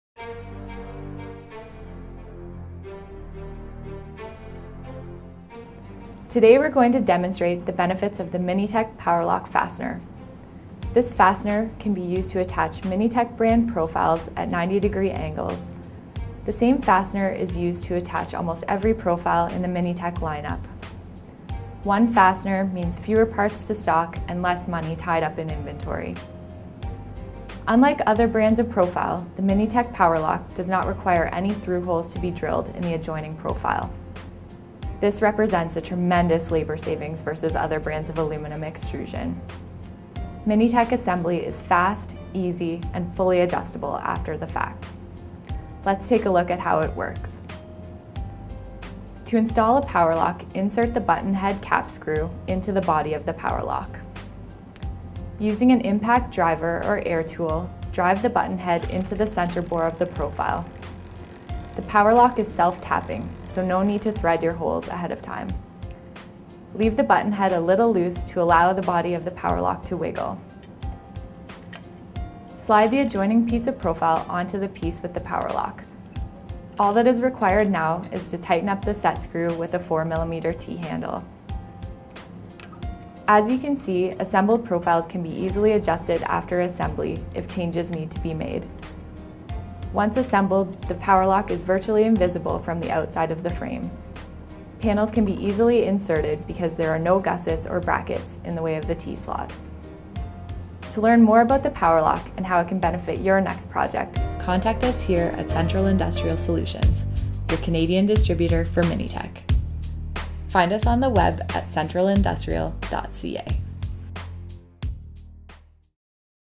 MiniTec Power Lock Demonstration
Watch how quickly and easily MiniTec profiles can be fastened using the MiniTec Power Lock fastener.